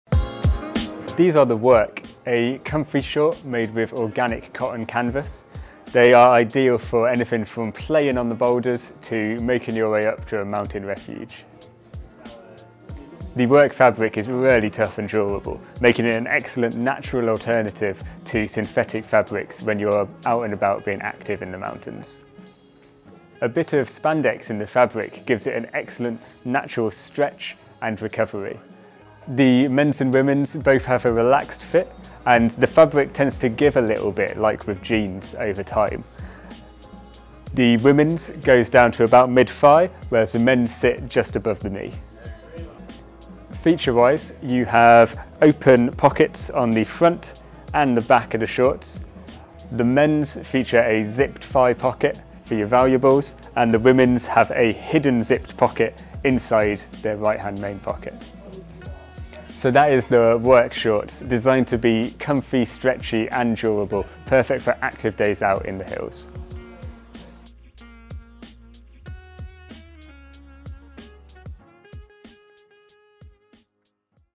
Alpkit Werk Organic Cotton Shorts
Durable, comfy, and technical organic cotton canvas shorts for an active mountain lifestyle